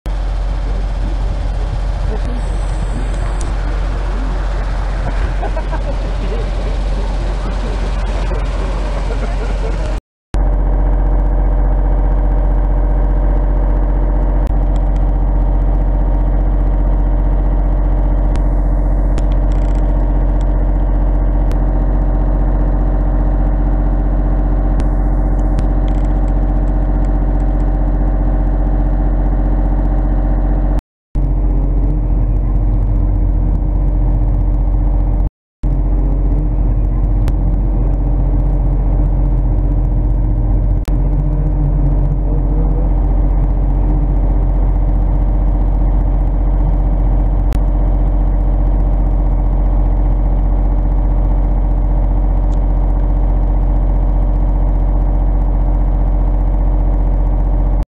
Team 5 Arriving Happy Valley Goose Bay CAIN'S QUEST 2012
Older footage of one of the longest & toughest snowmobile races in the world. Playlist of Cain's Quest videos on this channel.